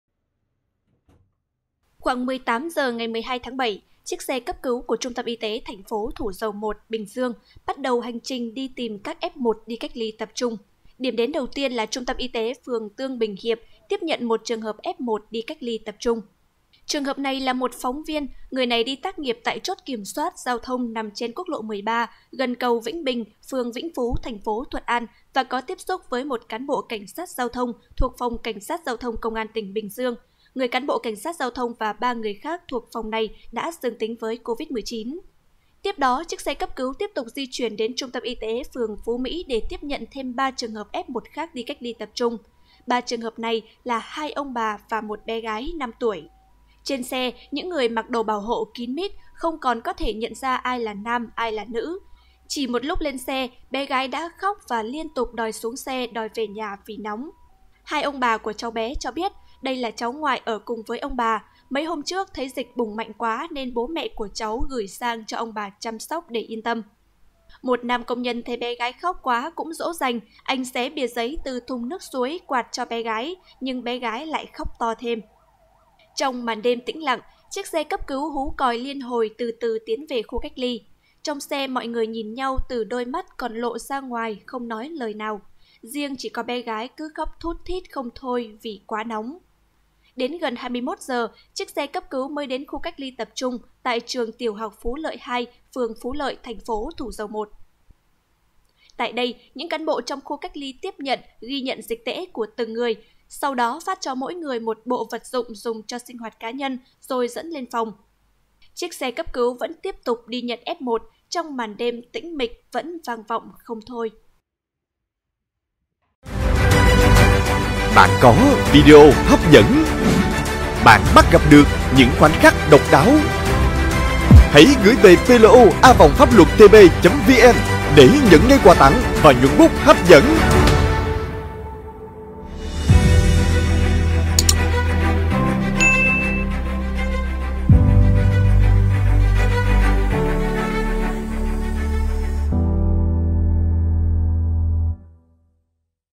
Bình Dương: Hành trình đi cách ly, bé gái 5 tuổi khóc đòi về - PLO
Bình Dương: Hành trình đi cách ly, bé gái 5 tuổi khóc đòi về
Khoảng 18 giờ ngày 12-7, chiếc xe cấp cứu của Trung tâm y tế TP Thủ Dầu Một (Bình Dương) bắt đầu hành trình tìm các F1 đi cách ly tập trung.